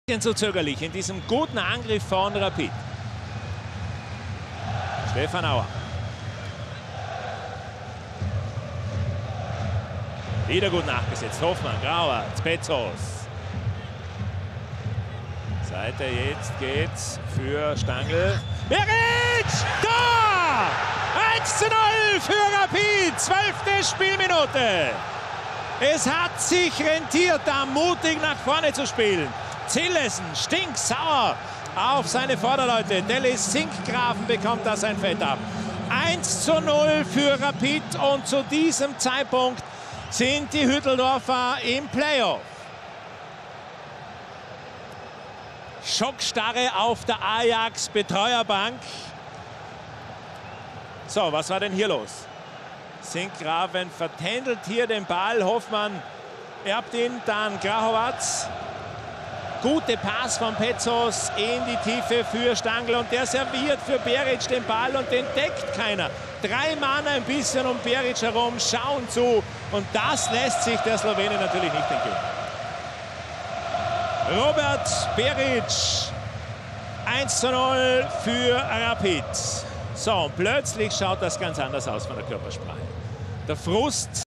Beric-Tor zum 1:0 in Amsterdam
So brachte Robert Beric den SK Rapid beim 3:2-Auswärtssieg gegen Ajax in der Amsterdam ArenA in Führung!